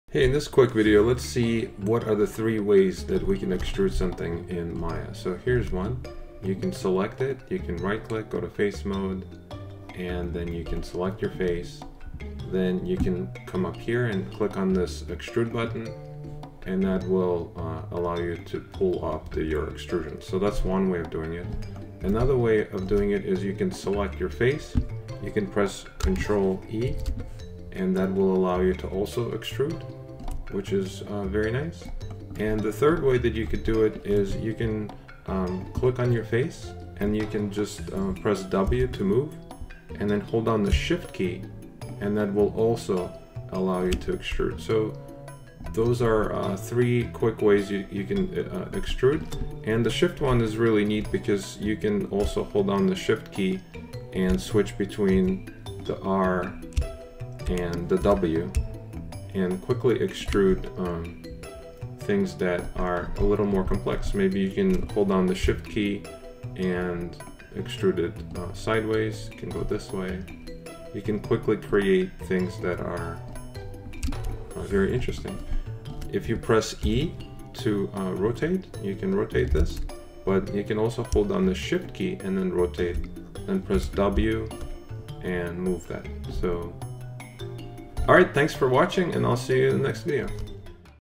3 Different Ways to EXTRUDE in Maya (Tips)
There are 3 different ways you can extrude your geometry during 3d modeling in Maya.